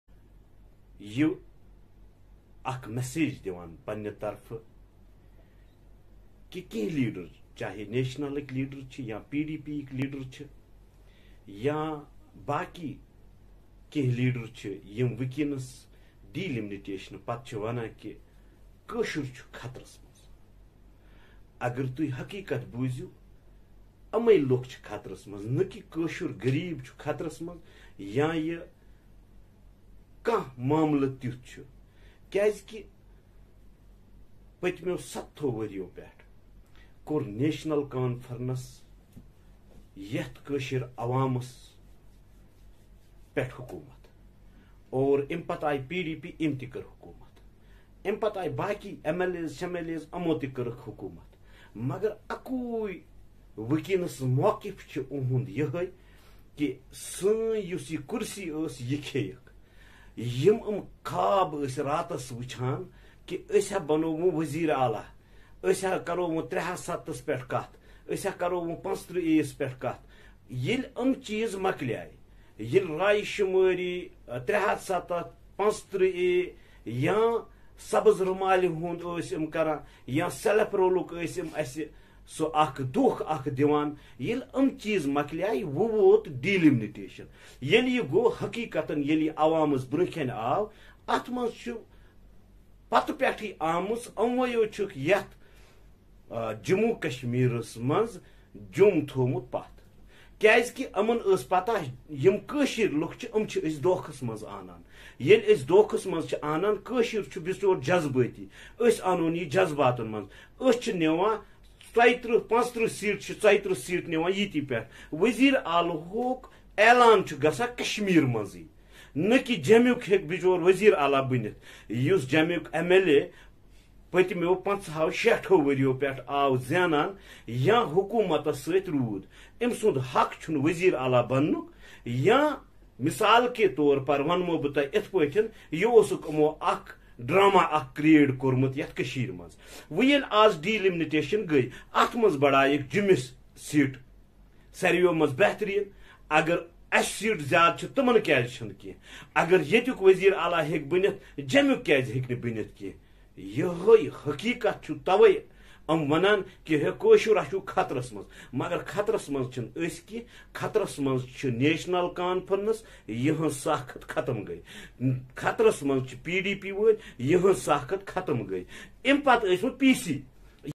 *Crocodile tears of NC, PDP for their chair not people: Javid Qurashi*ernment is committed to empowe
*Crocodile tears of NC, PDP for their chair not people: Javid Qurashi*

*Says, Modi Government is committed to empower people of Jammu Kashmir*

Handwara December 21:  Bhartiya Janta Party senior leader Javid Qurashi on Tuesday reacted to the rhetoric of NC, PDP for shedding crocodile tears over the Delimitation process.

Qurashi said, that the parties, like NC, PDP are upset about their chair not people. The two parties have ruined Jammu Kashmir all these year's he said.

He also said that the Modi Government is committed to empower people of Jammu and Kashmir irrespective of region, religion.
He asked the NC, PDP to stop shedding crocodile tears over Delimitation process. The people of Jammu Kashmir are no longer fool to fall in your trap he added.